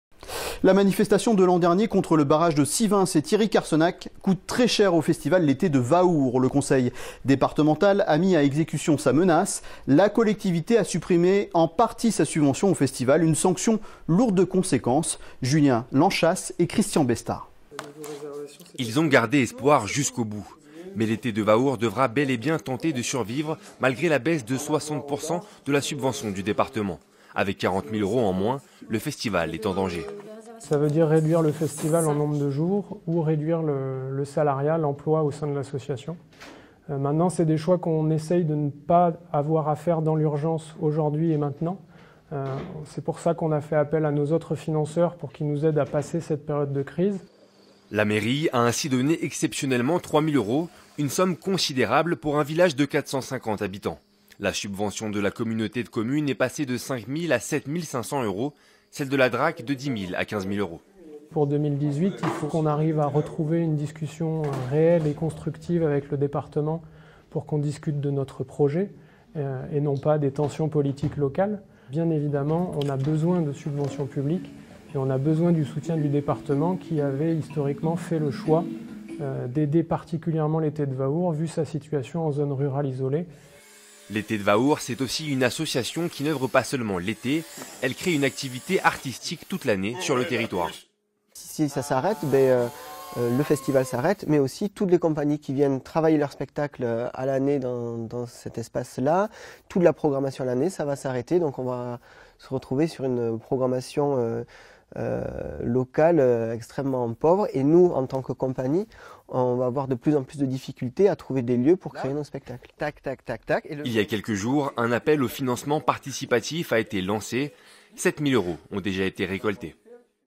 Prélude PPR - France 3 Tarn - Eté de Vaour
Reportage France 3 sur l'association l'été de Vaour, où nous étions en résidence pour le nouveau spectacle jeune public de la compagnie Sans Gravité, Prélude pour un poisson rouge.

Par Jérémy Philippeau, mise en scène Rémi Lasvènes. 45 minutes.